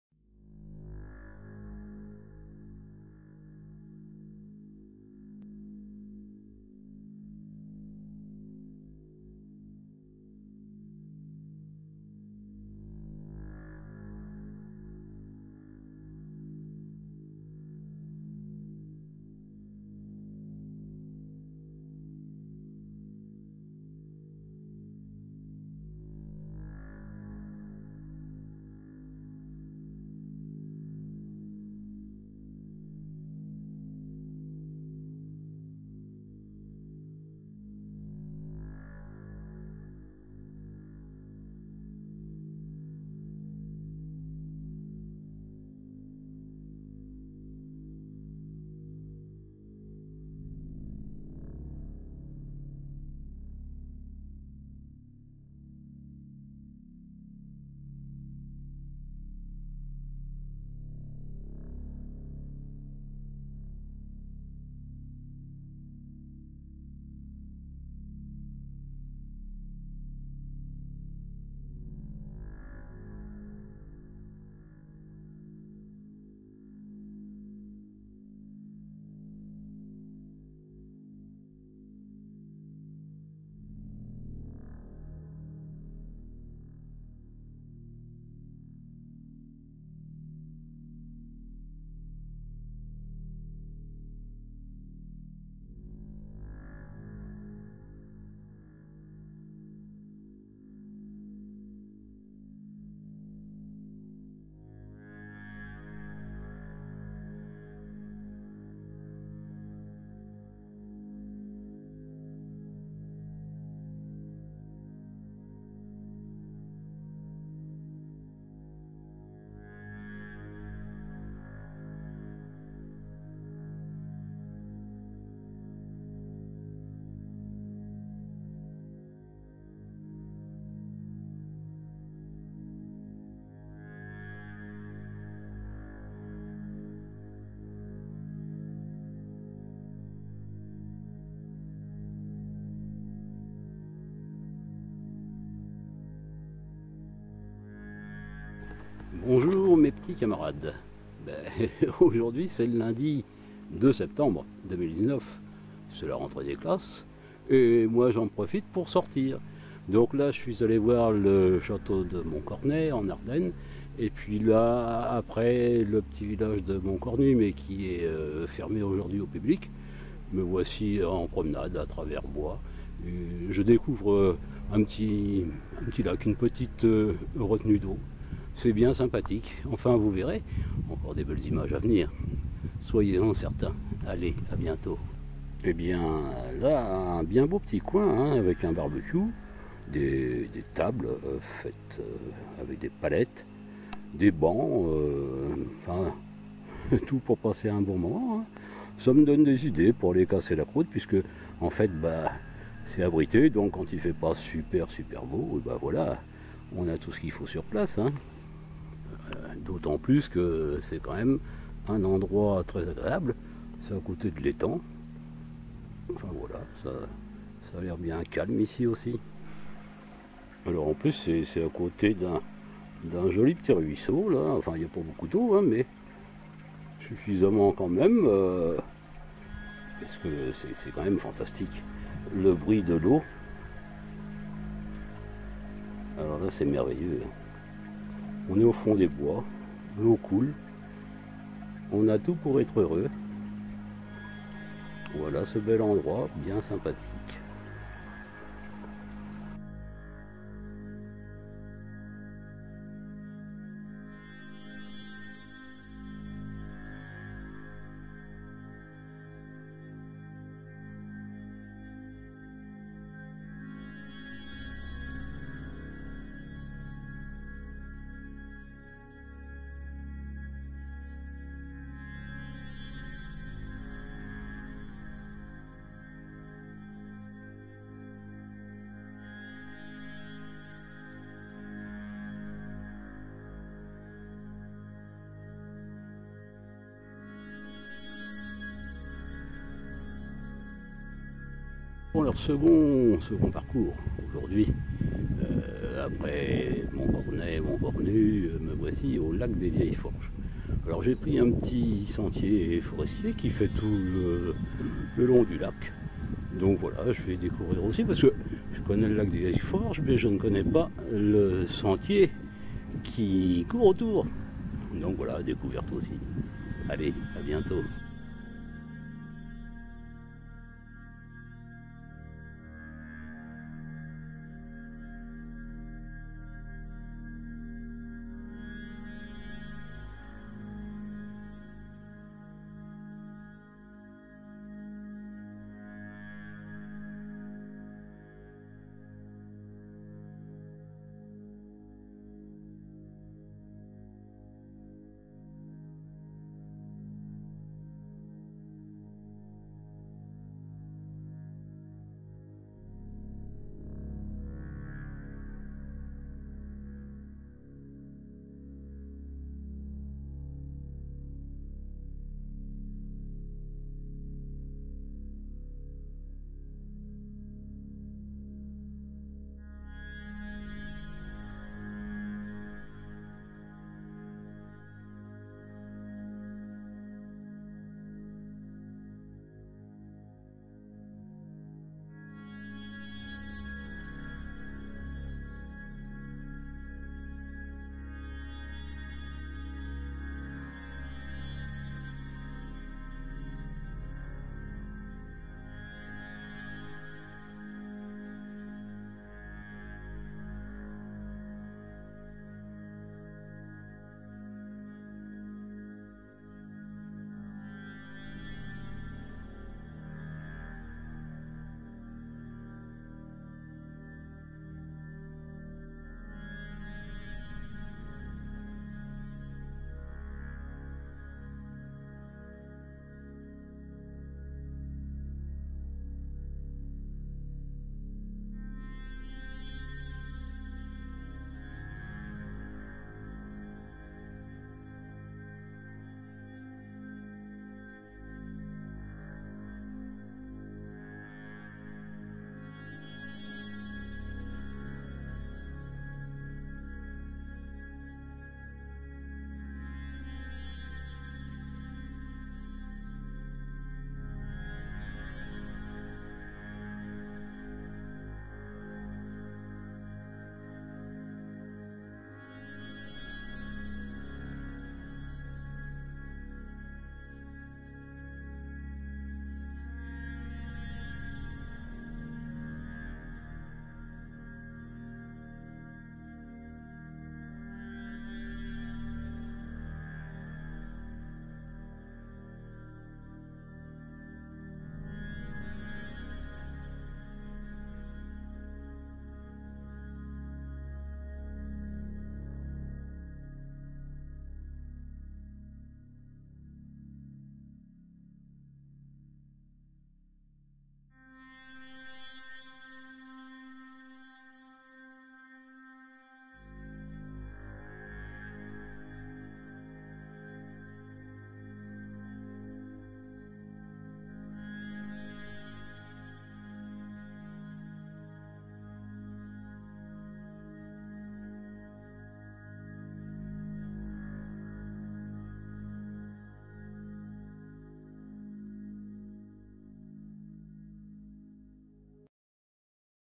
Moncornet, Mont Cornu et le Lac des Vieilles Forges
1er circuit : Randonnée à Montcornet en Ardenne, autour de l'Eglise et des ruines du Château, puis descente par le sentier pédestre vers le village médiéval du Mont Cornu avec visite de la Chapelle Marie-Madeleine qui se situe dans le cimetière.
2ème circuit : Randonnée sur le chemin forestier qui borde une partie du Lac des Vieilles Forges, dans le Département des Ardennes. De jolis paysages à découvrir !